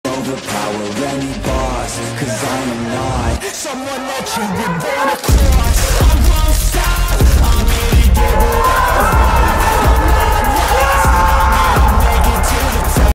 VERSTRAETEN 🇧🇪 When u win final u shout at like this.
FINAL FRA KHYAR vs BEL VERSTRAETEN. Antalya Grand Slam 2021, -60 k.